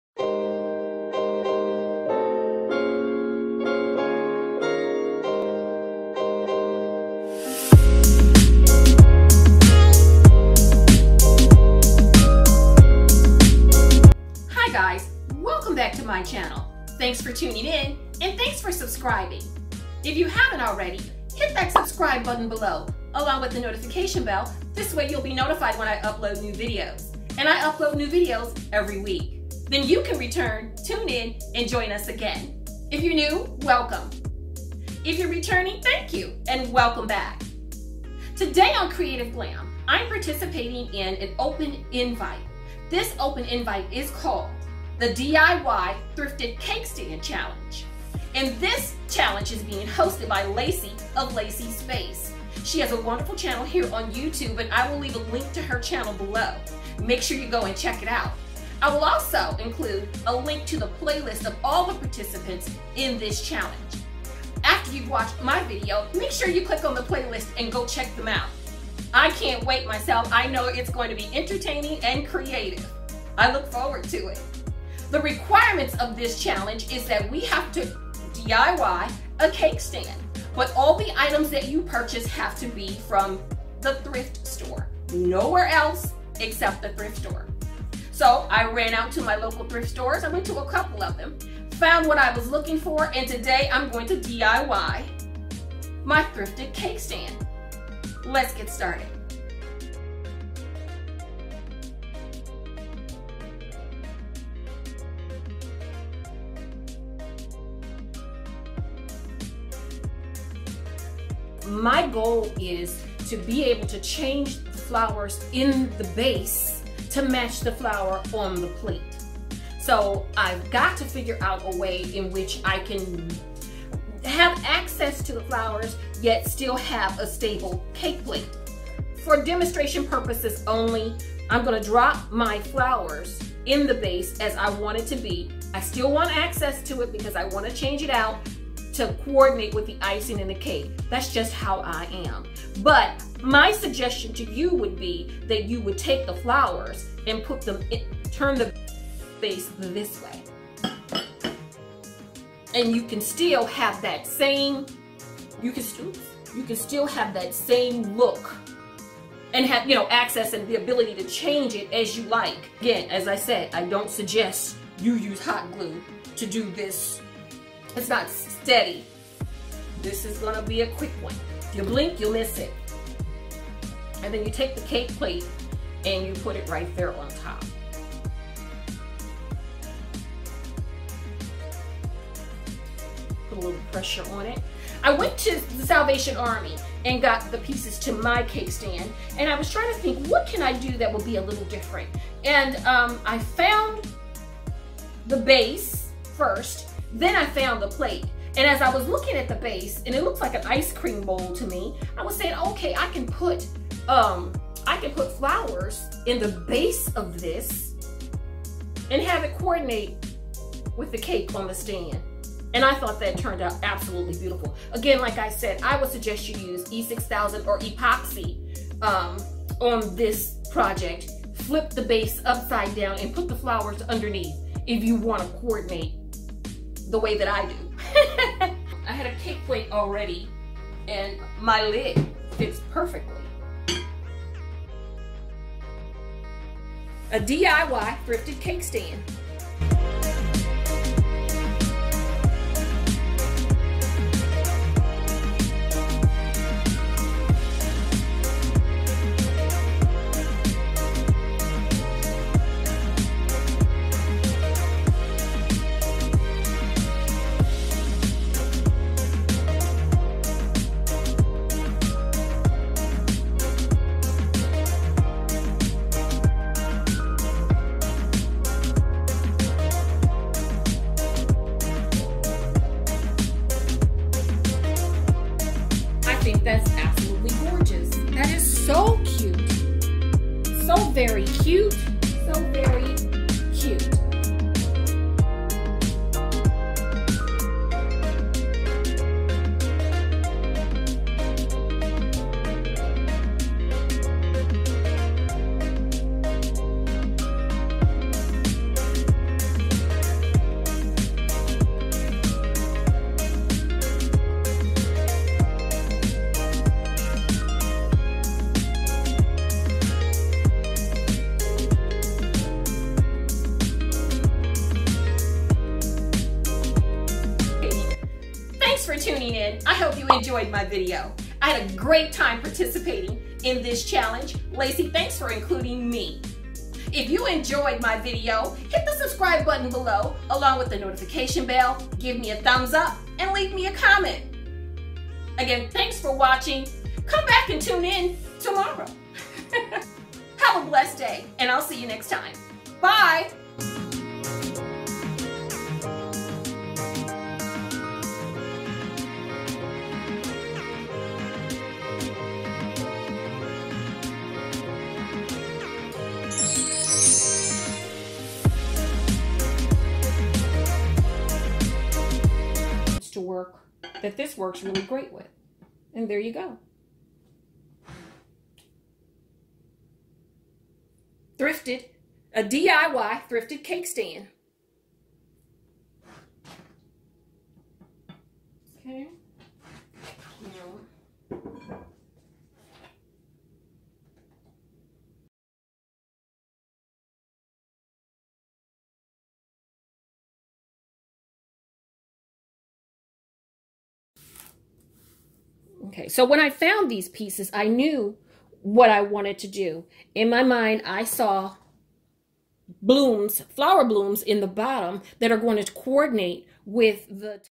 DIY - THRIFTED CAKE STAND CHALLENGE 🎂 Hosted By Lacee's Space ✨
Hi Guys! 💖 In this video, I am participating in a collaboration to DIY A THRIFTED CAKE STAND CHALLENGE.  Today I will share with you the items I purchased from the Salvation Army and my DIY CAKE STAND 2018.  😍

I love What I Do and I Do What I Love. 💖